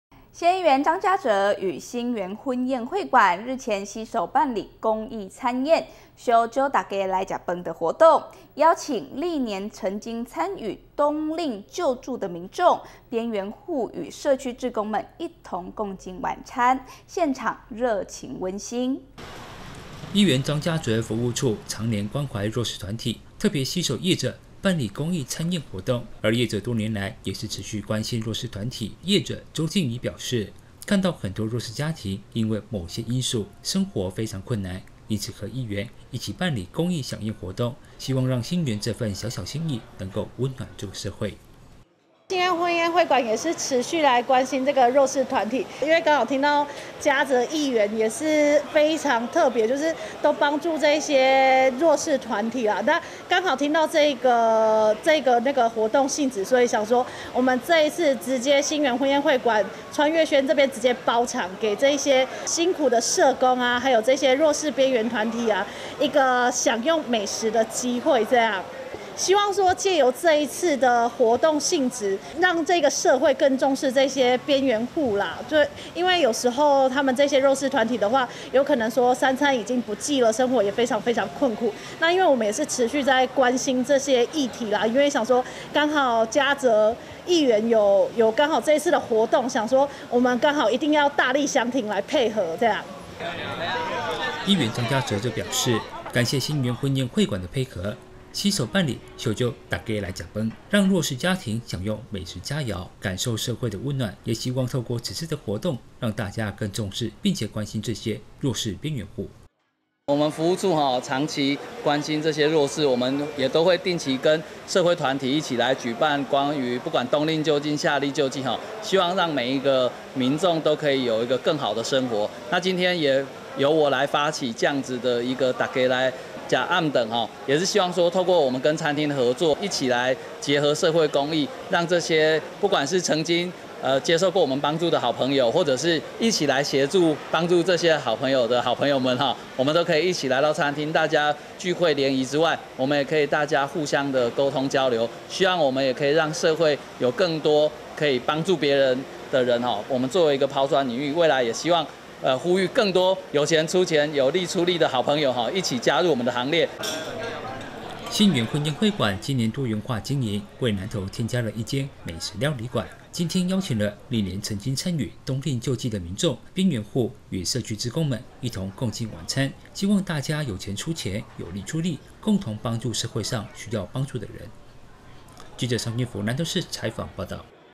做公益一起來 公益餐宴相揪來呷飯｜南投縣政新聞 20210331
縣議員張嘉哲服務處與心園婚宴會館，日前攜手辦理「公益餐宴～相揪大家來呷飯」活動，邀請歷年曾參與冬令救濟之民眾、邊緣戶與社區志工們，一同於心園婚宴會館川粵軒共進晚餐，現場熱情溫馨。